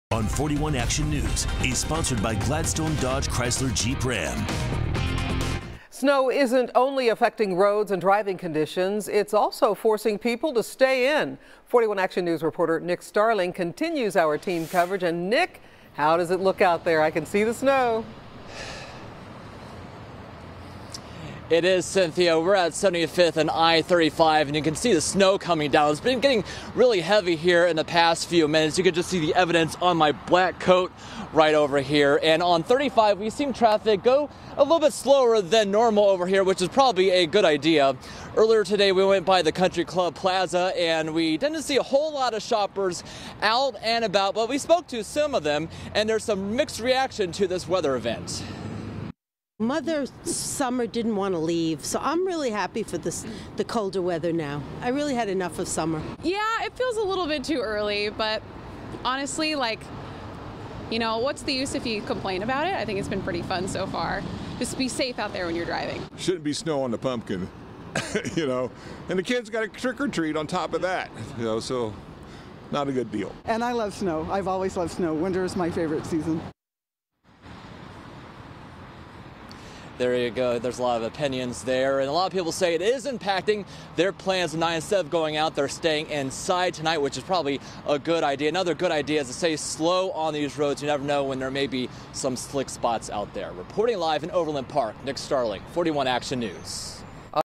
Kansas City's early snowfall impacting metro drivers, shoppers, businesses
Mother Nature dished out an October surprise for many people in the Kansas City area on Wednesday.